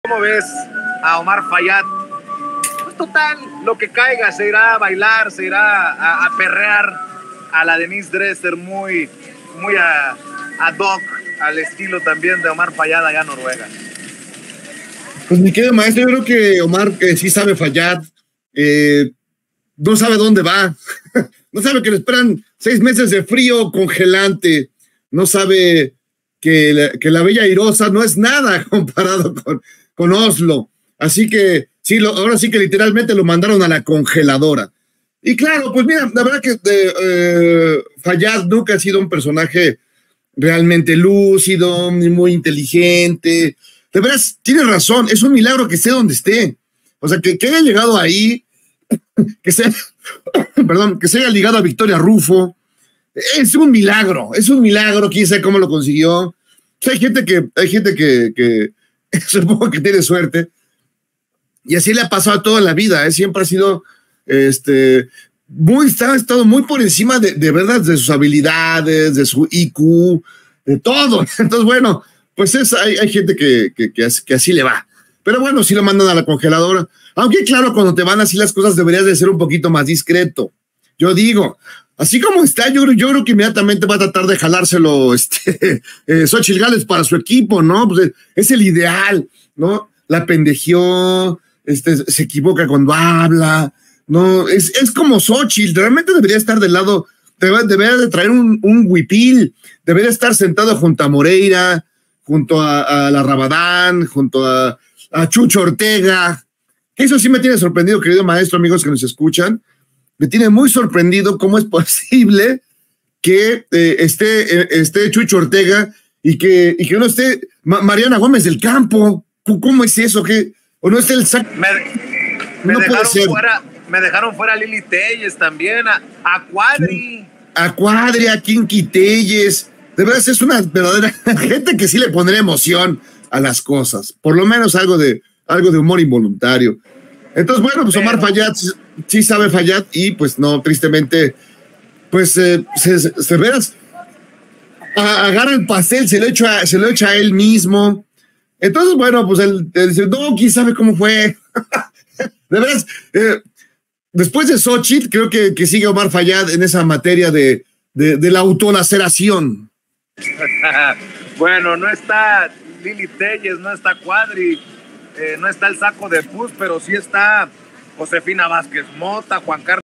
DESPUÉS DE XÓCHITL GÁLVEZ ¡SIGUE OMAR FAYAD EN EL TEMA DE LA AUTOLAC3R4CI0N!: JAIRO CALIXTO
Ante el nuevo nombramiento de Omar Fayad como Embajador de México en Noruega, el periodista Jairo Calixto aseguró que el ex Gobernador no tiene ni idea a lo que se enfrenta¡porque la bella airosa no es nada comparado con Oslo! Además, recalcó que, a pesar de la enorme suerte que se carga, es cómo Xóchitl Gálvez ¡e incluso reconoció que debería formar parte del equipo de quienes la apoyan!

Víctor Aramburu: Doctor en Políticas Públicas